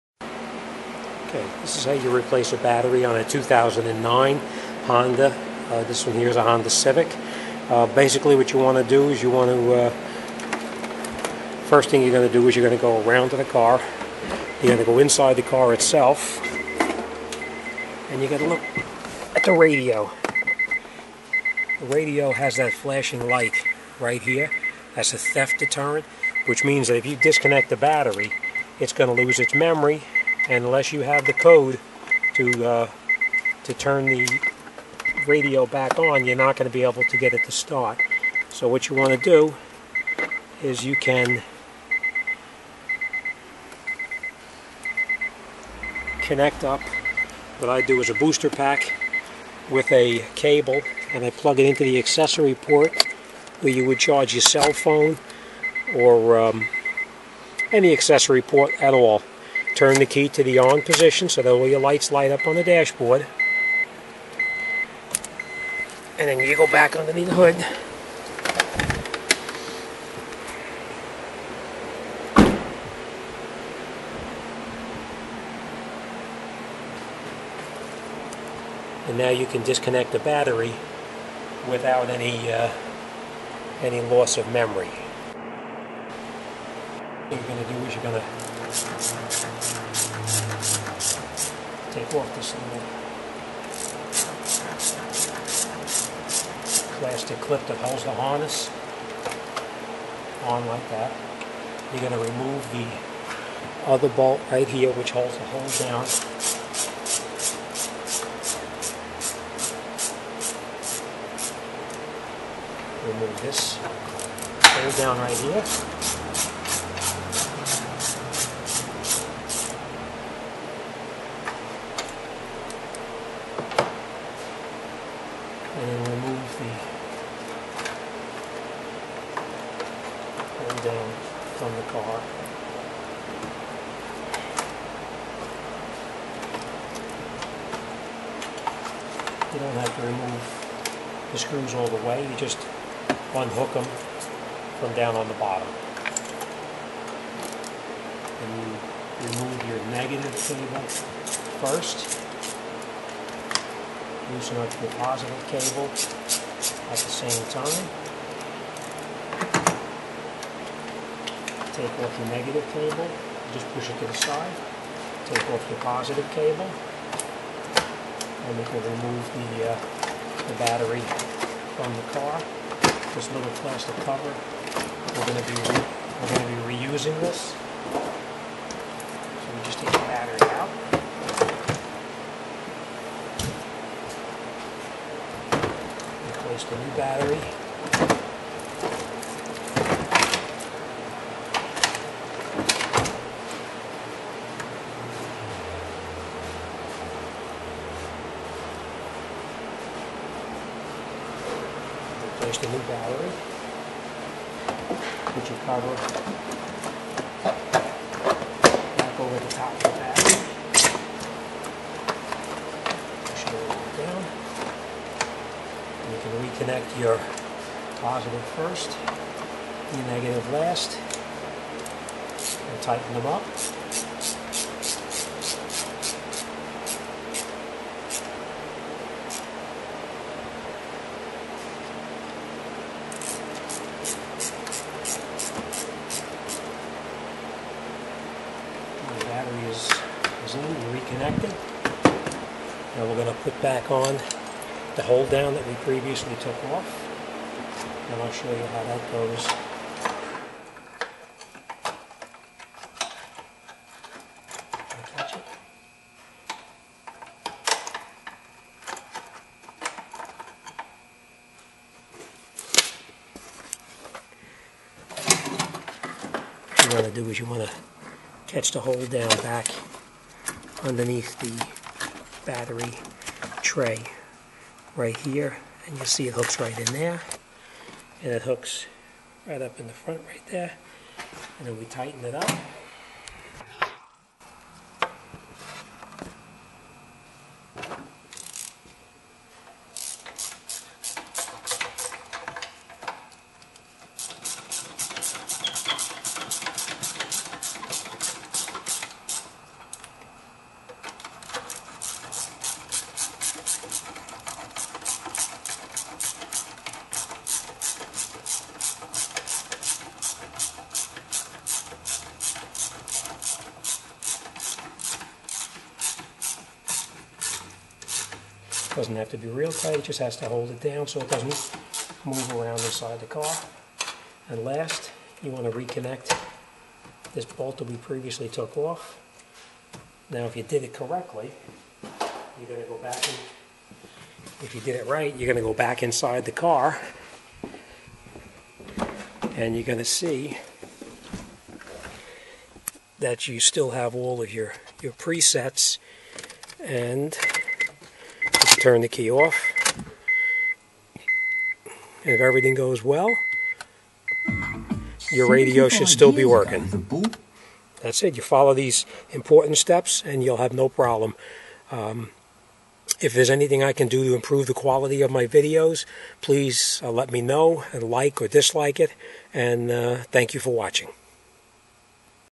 How To Replace The Battery On a 2009 Honda Civic
How To Replace The Battery On A 2009 Honda Civic Without Loosing The Radio Memory